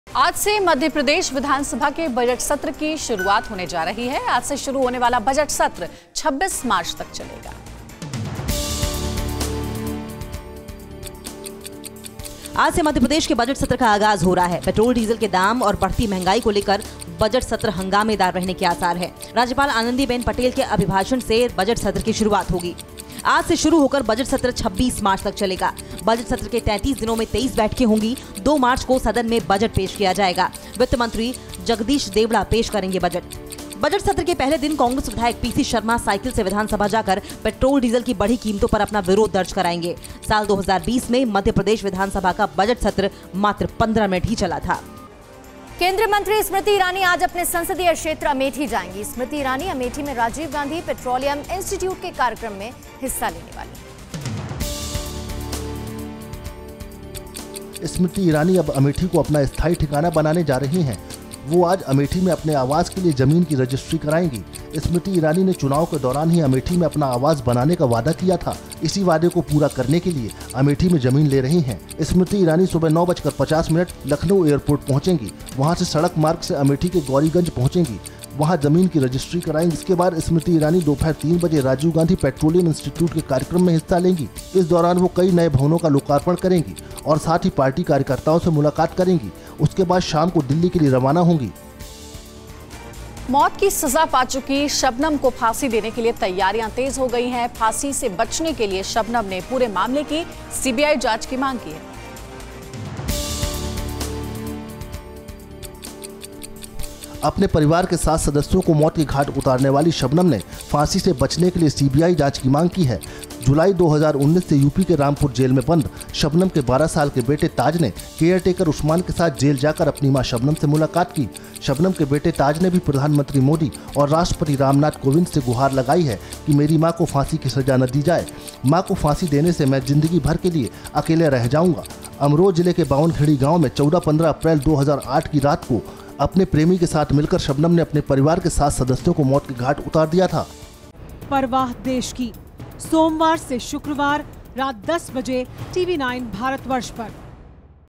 फांसी से बचने के लिए Shabnam का नया दांव, CBI जांच की मांग
मौत की सजा पाई शबनम को फांसी देने के लिए तैयारियां तेज हो गई है. फांसी से बचने के लिए शबनम ने पूरे मामले की सीबीआई जांच की मांग की है.

Credit : AadhaMinutePooriKhabar​ | PoornimaMishra​ Anjali​